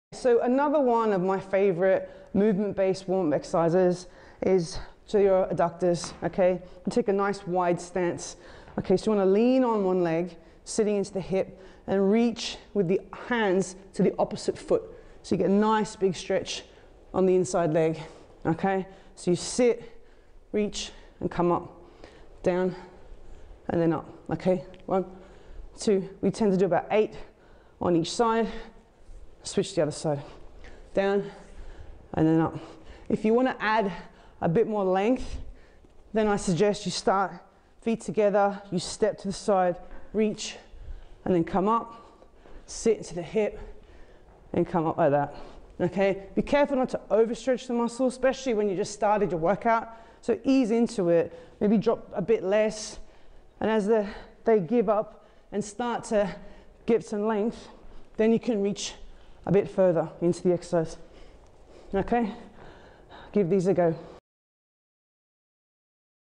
Technique Dynamic Adductor stretch
Movement based stretch that hits your adductors